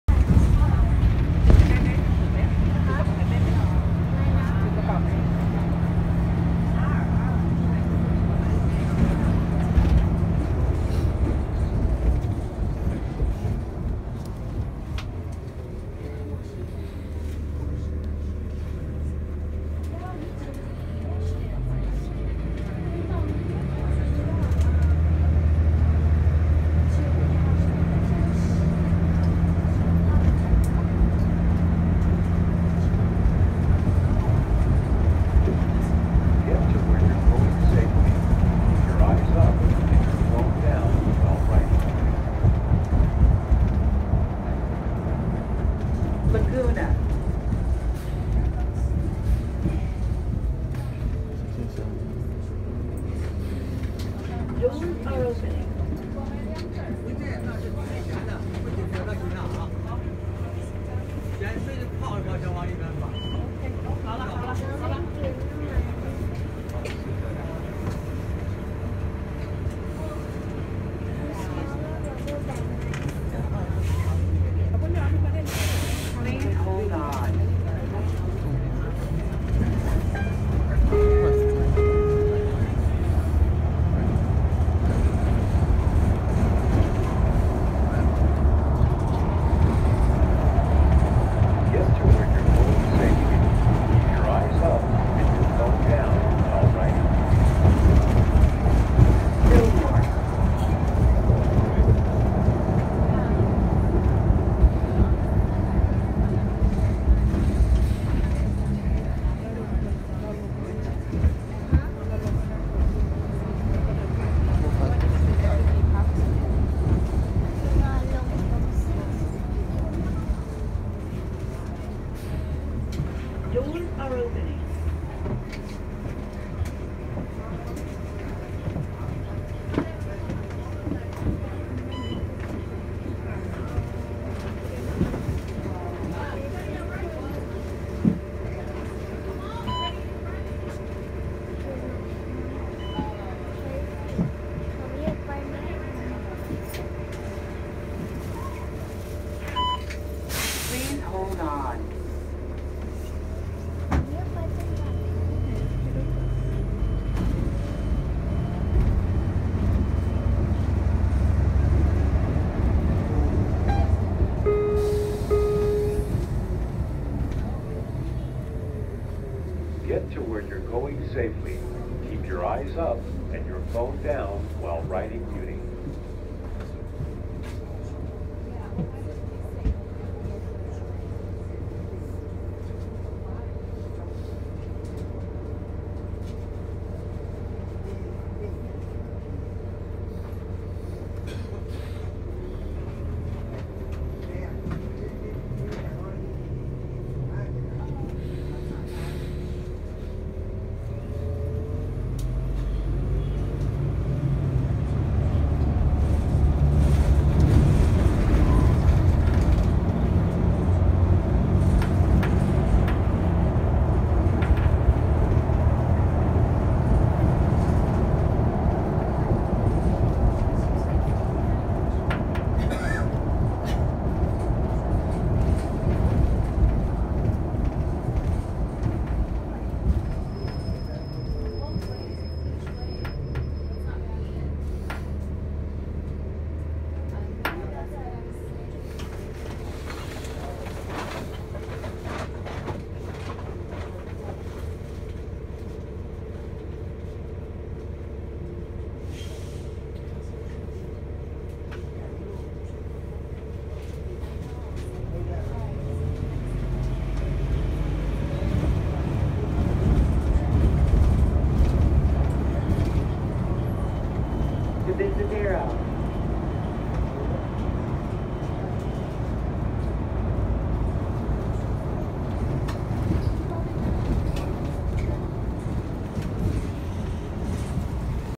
SF Muni 6679
Here is a bus ride that I took on line 38R to Lands End